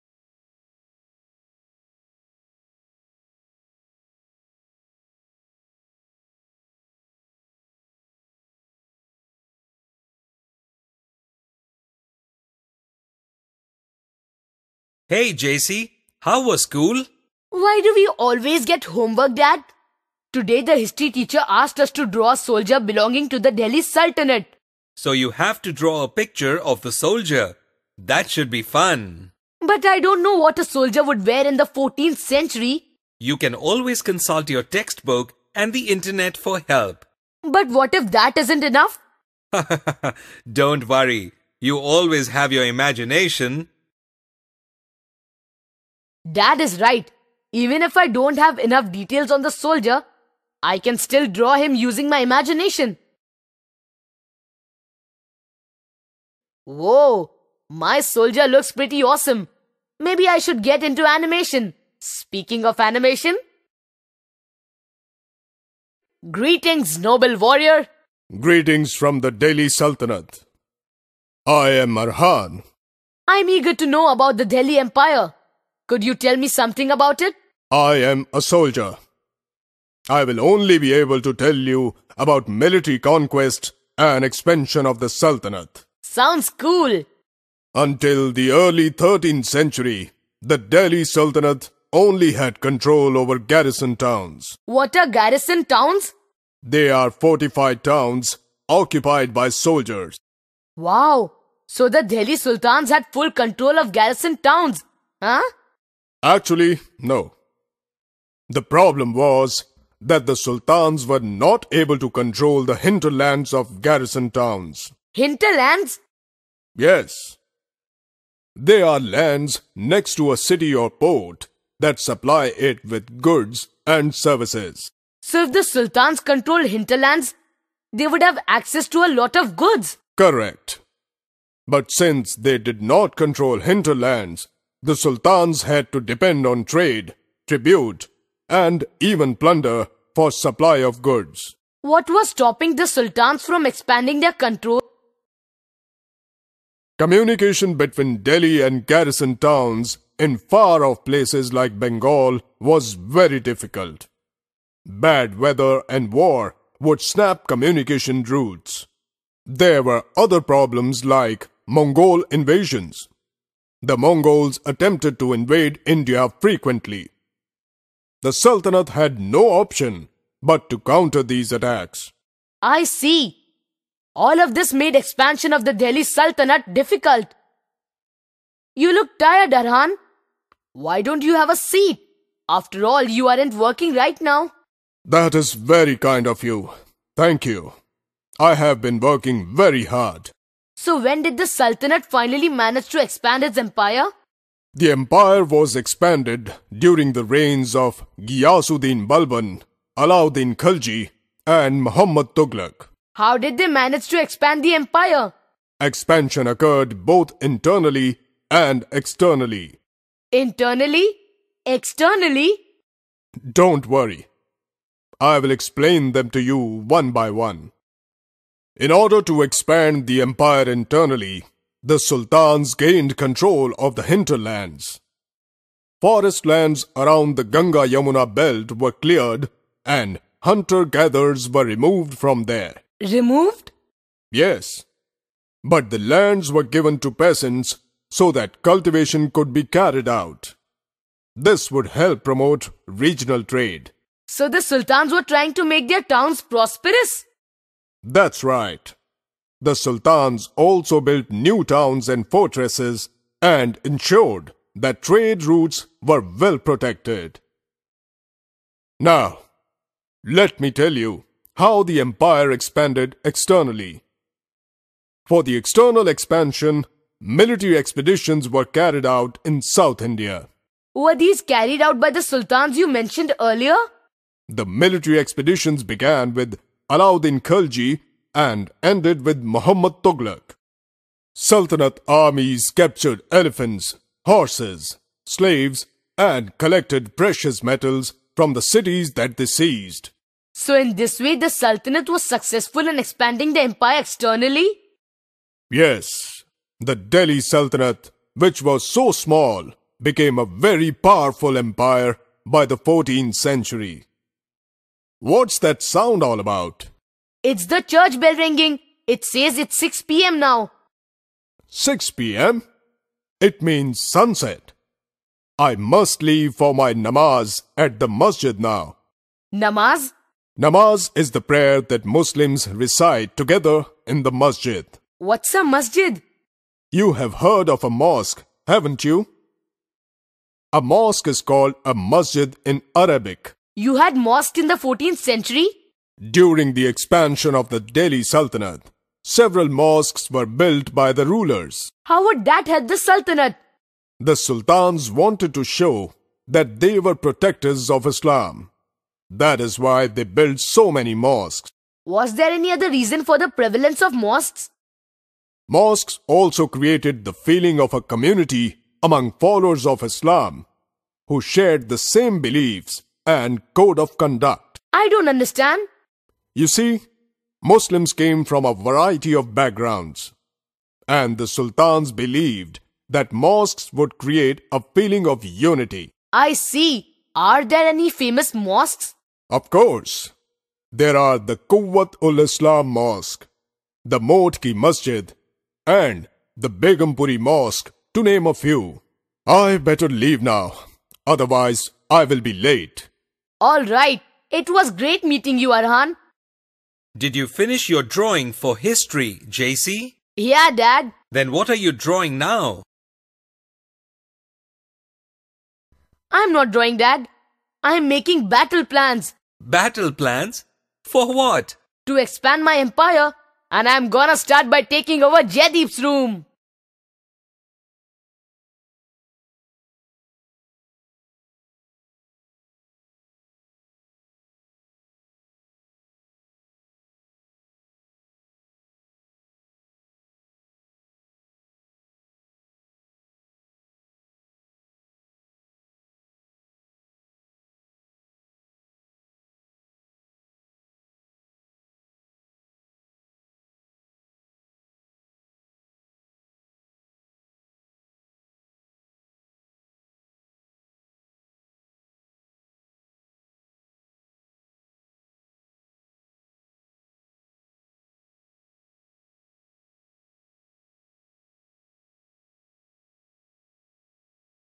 Expansion of the Delhi Sultanate
The audio-visual talks about the Delhi Sultans, their administration, expansion tactics and their places of worship.